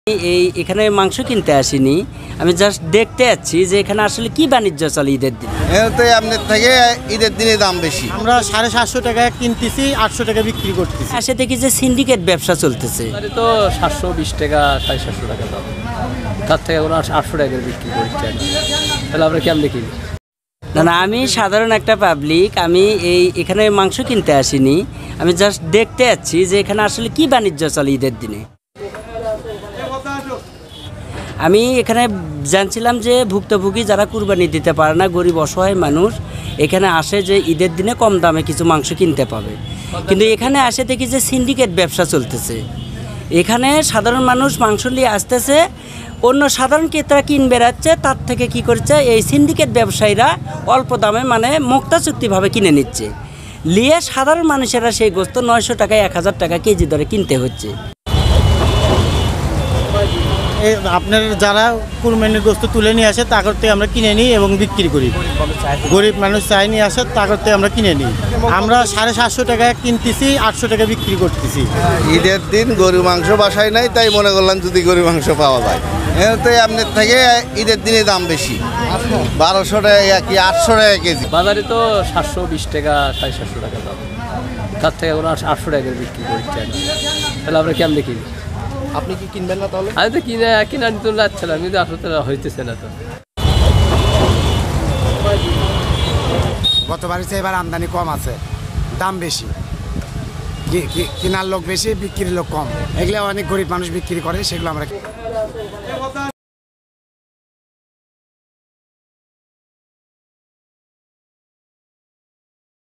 বগুড়ায় গরীবদের ঈদের মাং'স বিক্রিতে সি.ন্ডি.কেটের অ.ভি.যো.গ | Bogura | Daily Karatoa
The Daily Karatoa is one of the most popular Print Media in Bangladesh